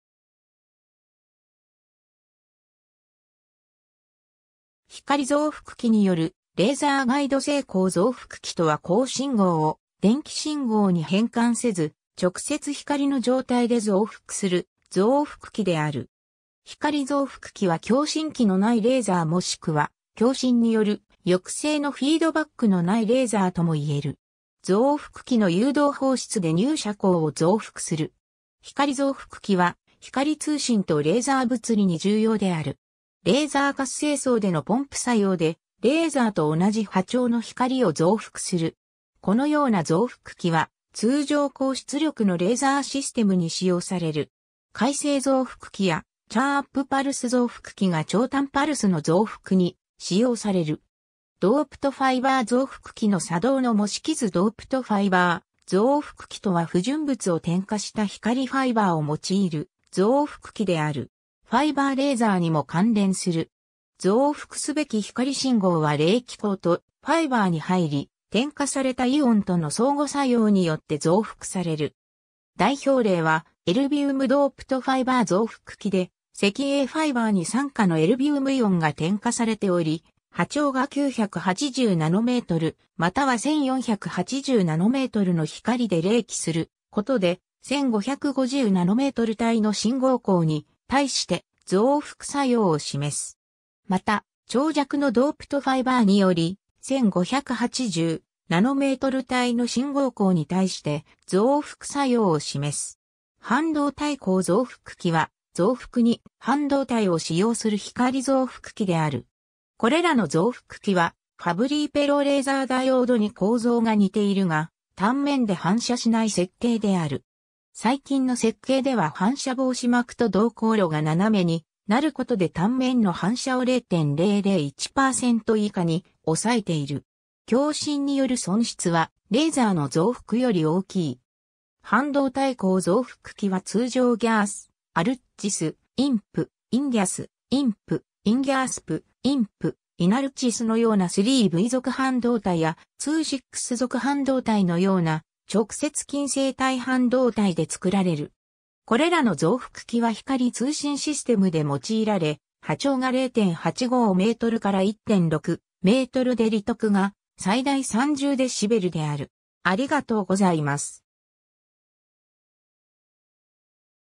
光増幅器
光増幅器によるレーザーガイド星 光増幅器（ひかりぞうふくき）とは光信号を電気信号に変換せず、直接光の状態で増幅する増幅器である。
光増幅器は共振器のないレーザーもしくは共振による抑制のフィードバックのないレーザーともいえる。
増幅器の誘導放出で入射光を増幅する。
光増幅器は光通信とレーザー物理に重要である。
 レーザー活性層でのポンプ作用でレーザーと同じ波長の光を増幅する。
このような増幅器は通常高出力のレーザーシステムに使用される。
回生増幅器やチャープパルス増幅器が超短パルスの増幅に使用される。
 ドープトファイバー増幅器の作動の模式図 ドープトファイバー増幅器（DFAs）とは不純物を添加した光ファイバーを用いる増幅器である。
ファイバーレーザーにも関連する。
増幅すべき光信号は励起光とファイバーに入り、添加されたイオンとの相互作用によって増幅される。
代表例はエルビウムドープトファイバー増幅器（EDFA）で石英ファイバーに3価のエルビウムイオン（Er3+）が添加されており、波長が980 nmまたは1,480 nmの光で励起することで、1,550 nm帯の信号光に対して増幅作用を示す。
 また、長尺のドープトファイバーにより、1,580nm帯の信号光に対して増幅作用を示す。
 半導体光増幅器は増幅に半導体を使用する光増幅器である。
これらの増幅器はファブリーペローレーザーダイオードに構造が似ているが、端面で反射しない設計である。
最近の設計では反射防止膜と導光路が斜めになることで端面の反射を0.001%以下に抑えている。
共振による損失はレーザーの増幅より大きい。
 半導体光増幅器は通常 GaAs/AlGaAs、InP/InGaAs、InP/InGaAsP、InP/InAlGaAs のようなIII-V属半導体やII-VI属半導体のような直接禁制帯半導体で作られる。
これらの増幅器は光通信システムで用いられ波長が0.85 µm から1.6 µm で利得が最大30dBである。